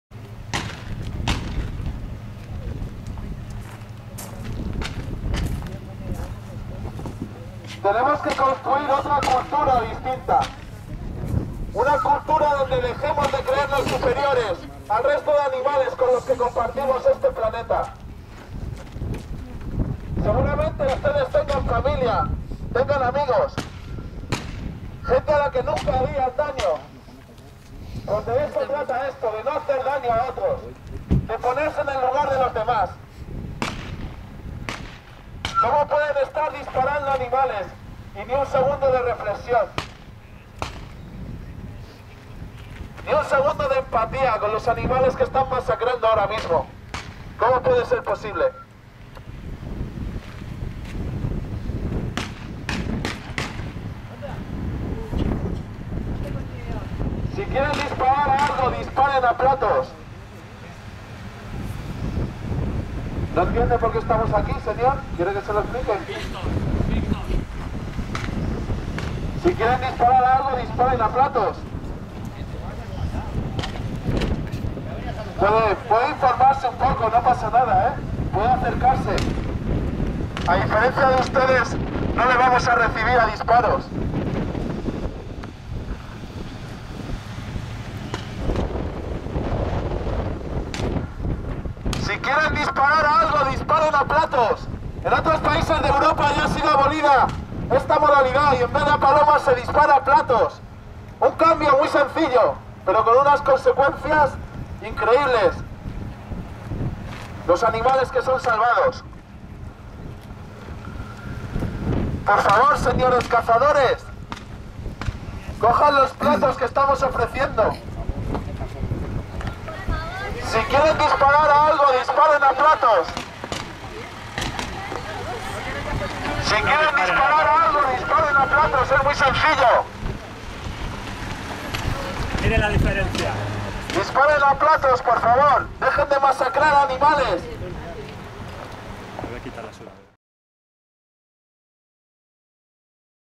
IN SITU: "Protesta contra el campeonato de tiro a palomas"
Sigue nuestros programas y noticias en TVAnimalista.com

Activistas de Igualdad Animal y Equanimal han salido de nuevo a protestar contra la brutal práctica de disparar a cientos de palomas por diversión.

Este pasado domingo 18 de marzo, plantándose frente a la puerta del club de tiro de Villarejo de Salvanés (Madrid) durante uno de los campeonatos, ofrecieron a los participantes platos de tiro, para que dejen de matar animales, tal como se viene haciendo ya en muchos países.